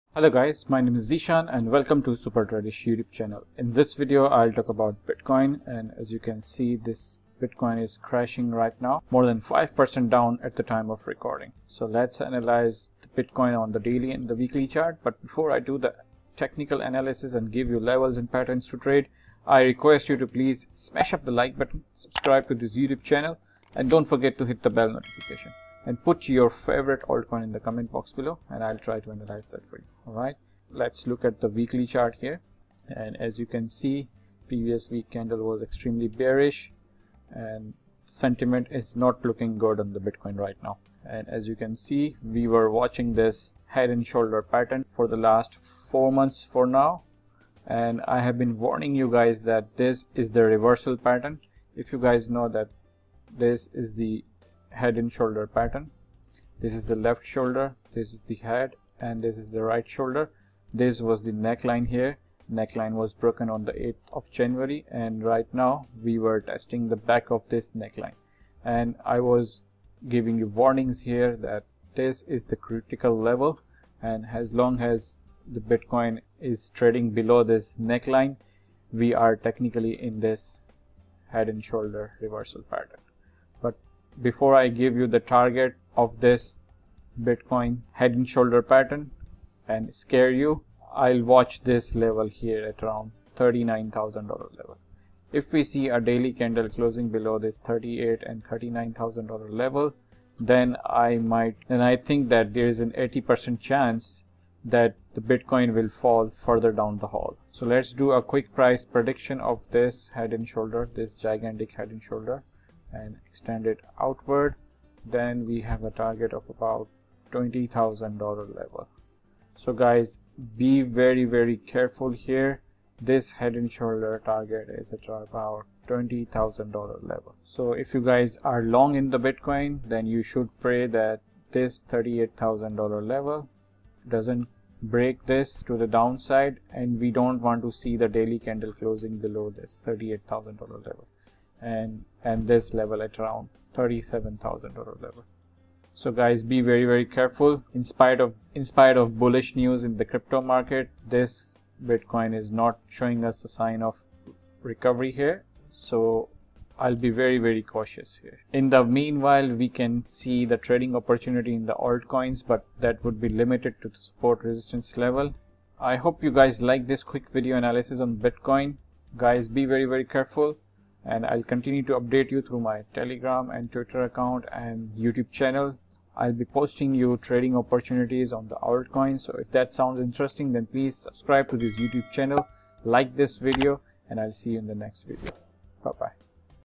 Bitcoin BTC Price Prediction Urgent Alert!
This video is about Bitcoin BTC Price Prediction in which I talked about head and shoulders pattern on daily chart of Bitcoin